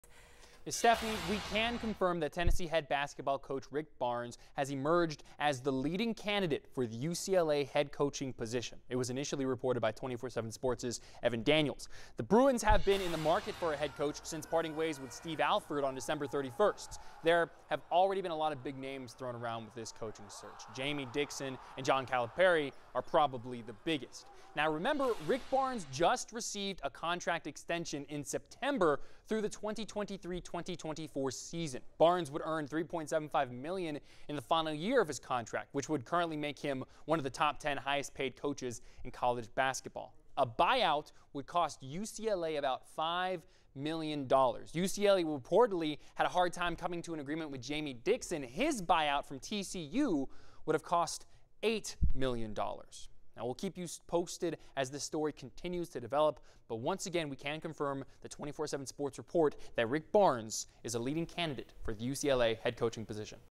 Source: Barnes leading candidate for UCLA head coach job
The Bruins have been in the market for a head coach since parting ways with Steve Alford on Dec. 31.